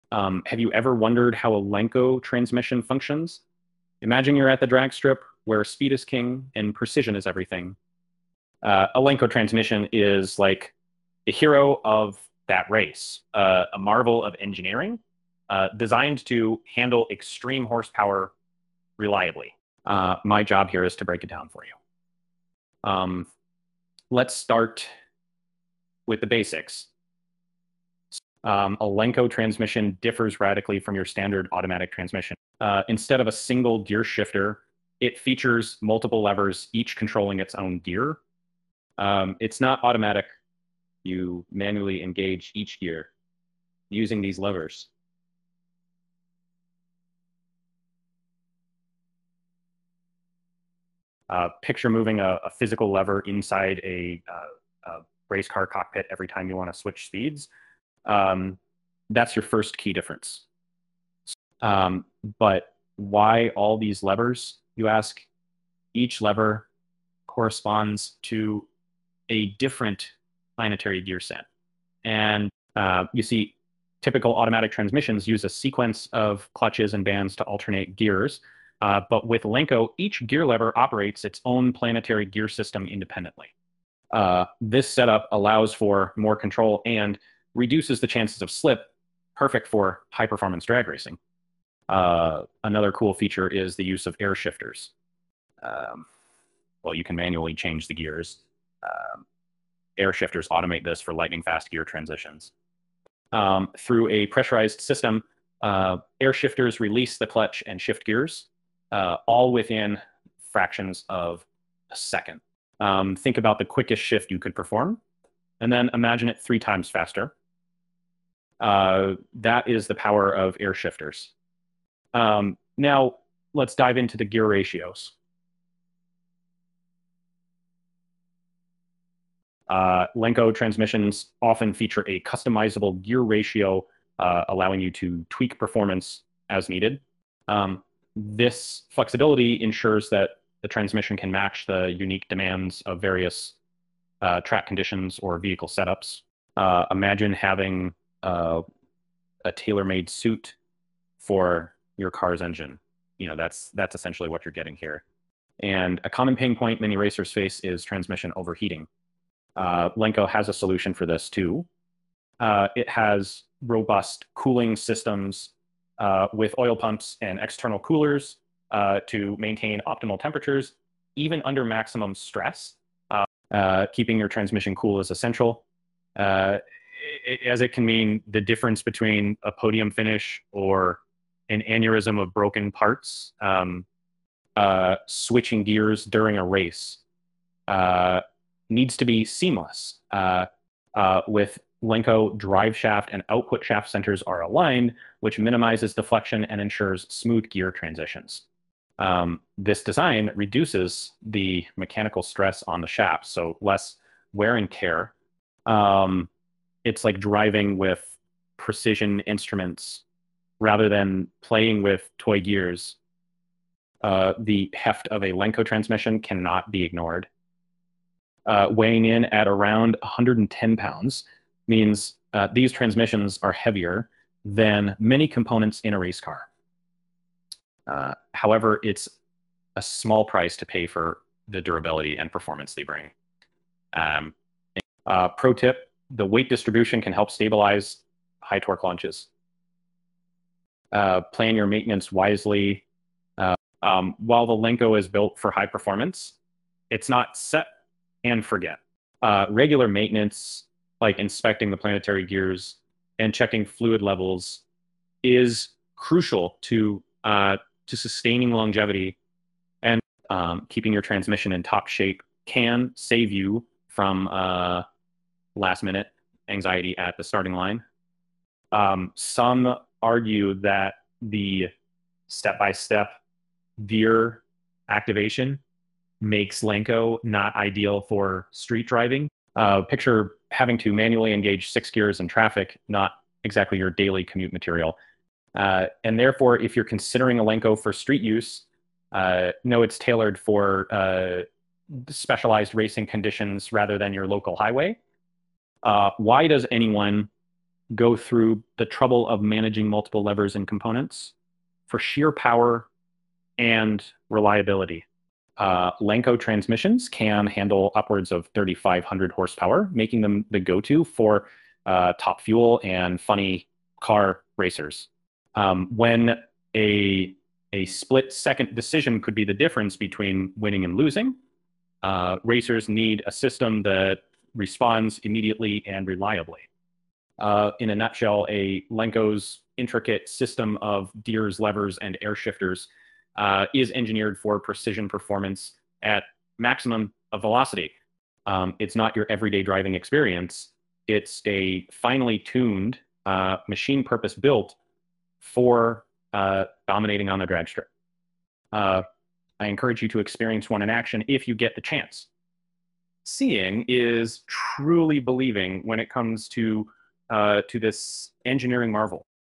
How Does A Lenco Transmission Work?
Dive into the fascinating world of Lenco transmissions, the true heroes of the drag strip. This video unravels how these engineering marvels handle extreme horsepower with precision and reliability. Unlike standard automatic transmissions, Lenco uses multiple manual levers for unparalleled control and reduced slip, perfect for high-performance racing. Discover the magic of air shifters for lightning-fast gear changes and customizable gear ratios to tailor your car's performance. With robust cooling systems and aligned drive components, Lenco ensures smooth transitions and durability, even at full throttle. Though heavier than typical transmissions, their performance and power handling capacity make them indispensable for top racers. Note: they’re built for the track, not your daily commute. Watch and learn why Lenco is the go-to choice for those who demand the best in precision racing technology.